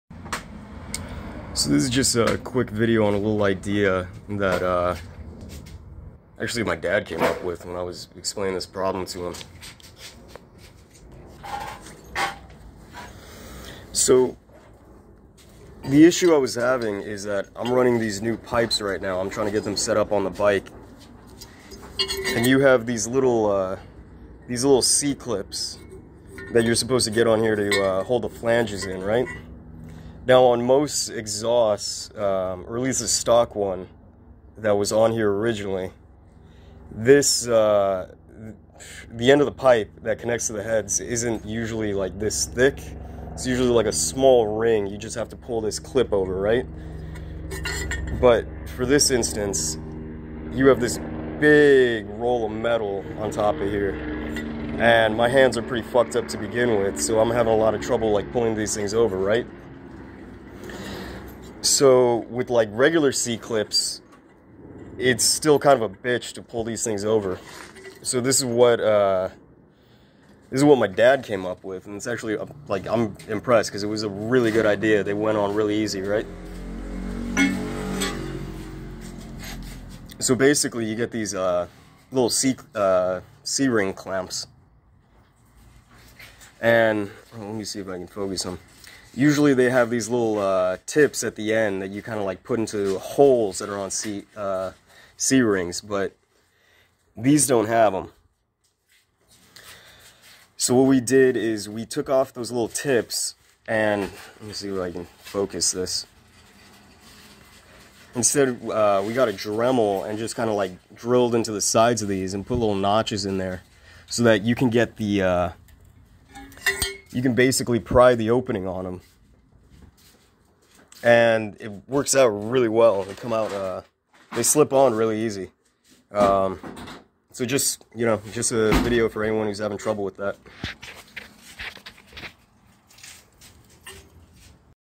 Helpful idea for removing or reinstalling Harley exhaust flange clips.
Just a tip, for anyone who's having trouble with the iron clips on Harley exhaust flanges.